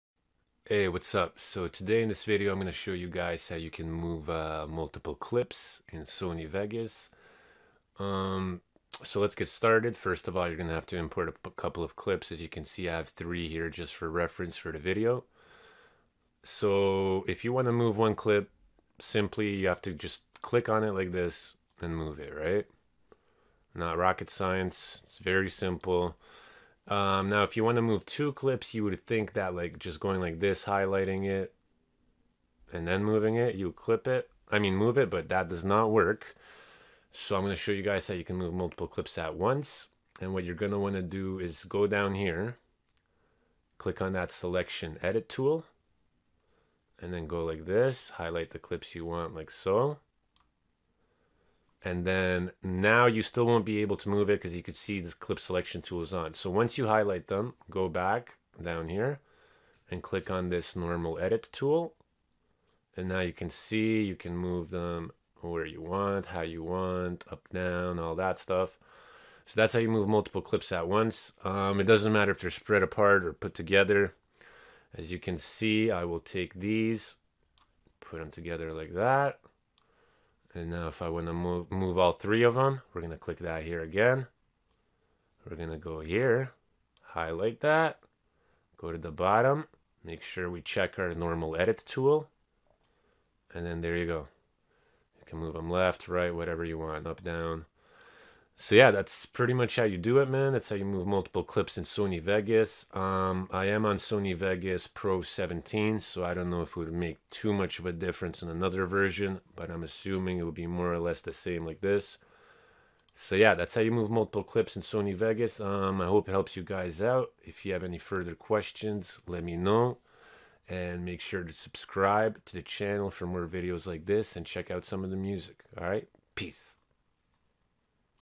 How to Move Multiple Clips in Sony Vegas
Quick video showing how to move multiple clips in sony vegas. Hope it helps and make sure to sub for more videos like this!

Move Multiple Clips Sony Vegas
Sony Vegas how to move multiple clips
sony vegas move multiple clips